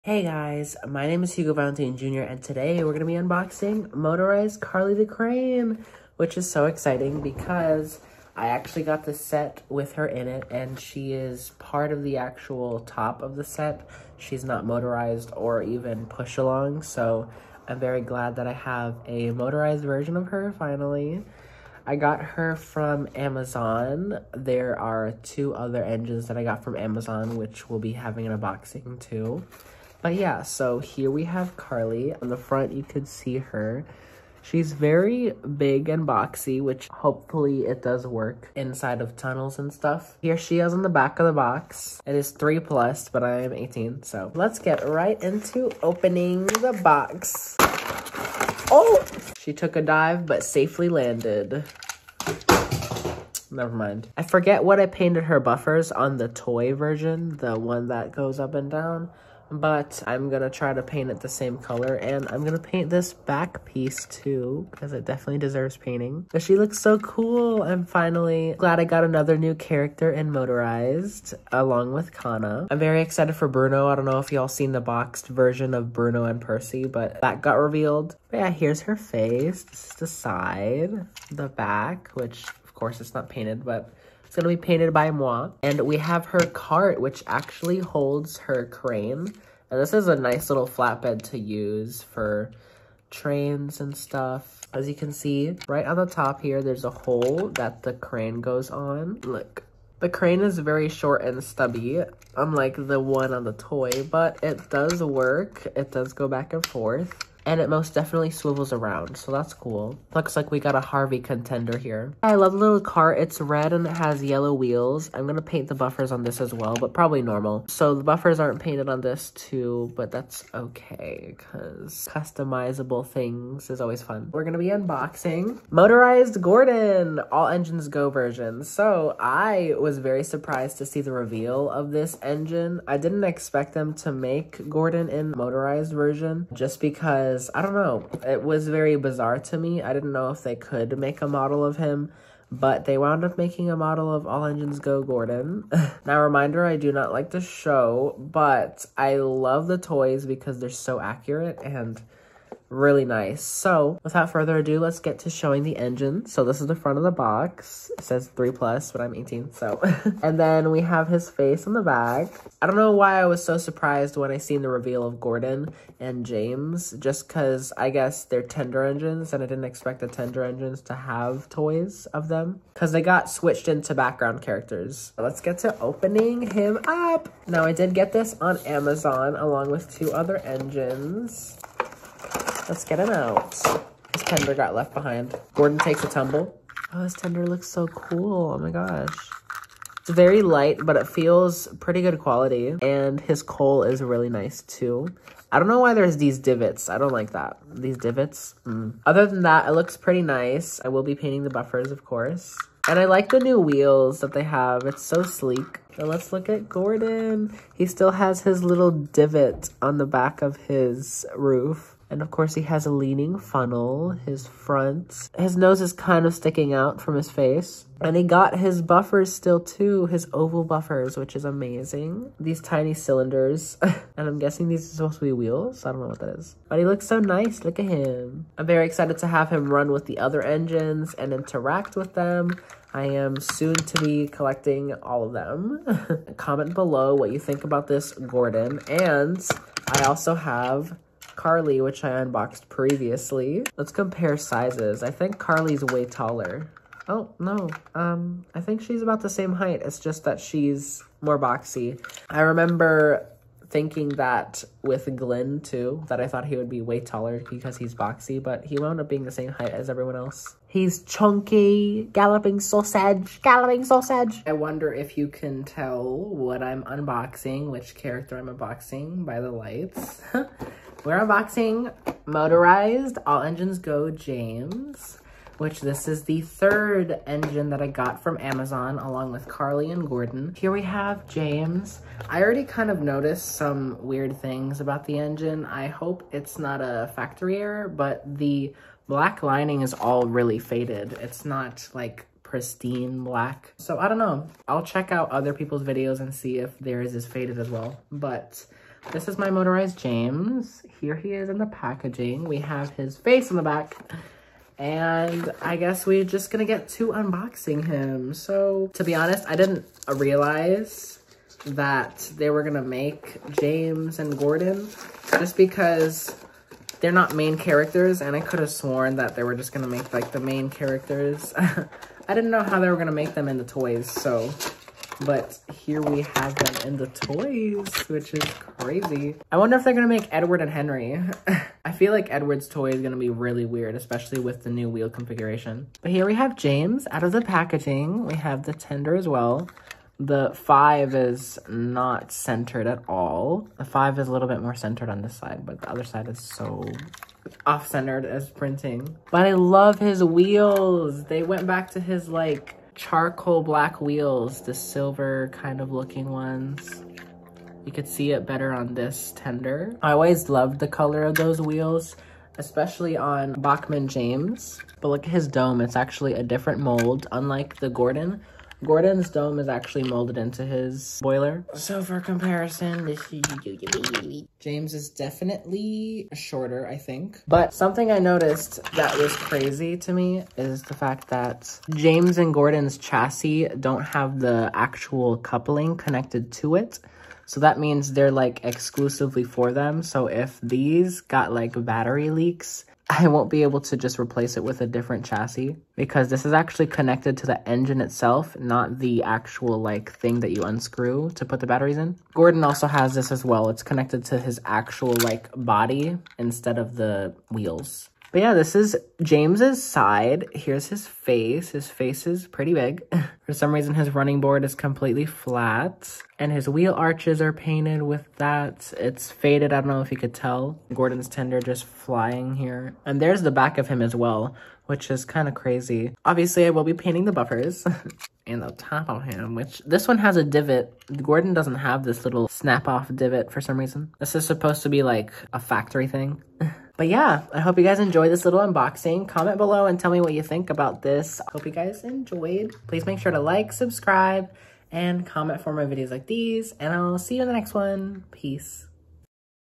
Motorized Carly, Gordon, & James Unboxing! {All Engines Go!}
In this unboxing video, I open up three All Engines GO variants of (now) side characters. I compare the sizes, show little details, and give my opinion on them all. Hope you enjoy!

Get these engines from Amazon below! (AD)
Motorized Carly:
Motorized Gordon:
Motorized James: